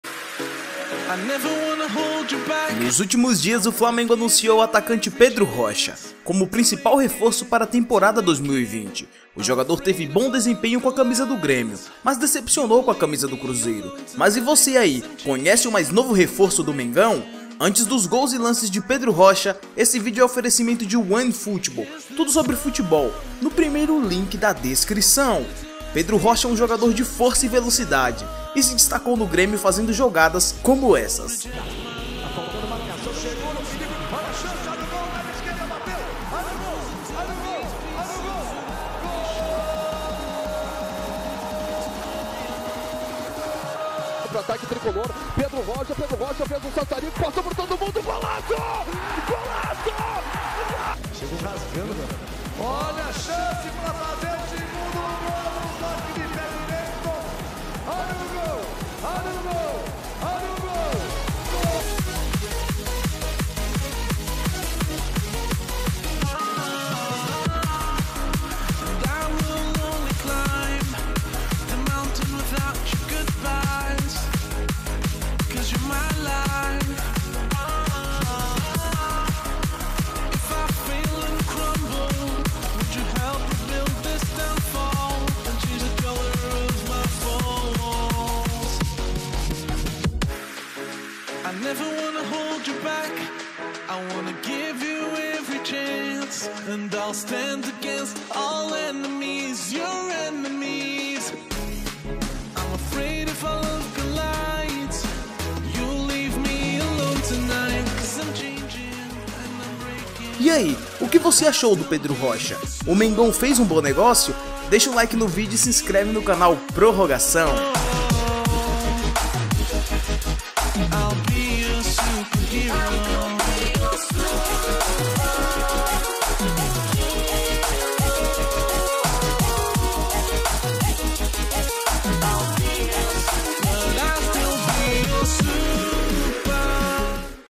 CONHEÇA PEDRO ROCHA NOVO REFORÇO DO FLAMENGO
Baixe Grátis o ONEFOOTBALL para ANDROID e IOS!!

conheça a nova contratação do Flamengo para a Temporada 2020!